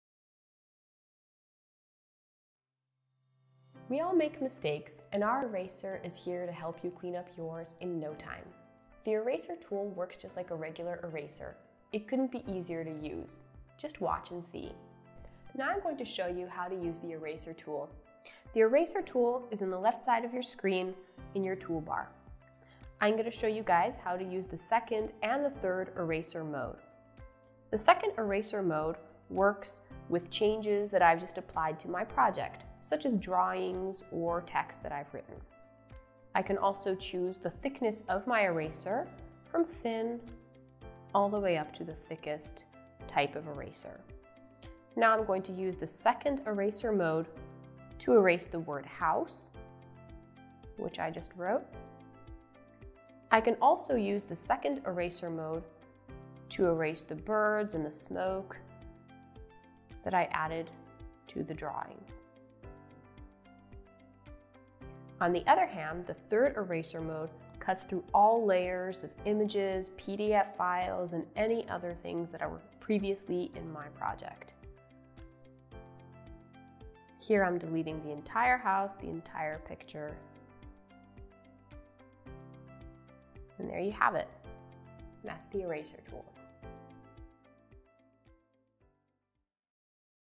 The Eraser
Learn to use the Eraser Tool to fix any mistake you make in seconds.

Check out our brand new Essentials playlist to find out how to get started using Explain Everything and make your remote lessons and meetings that much more engaging!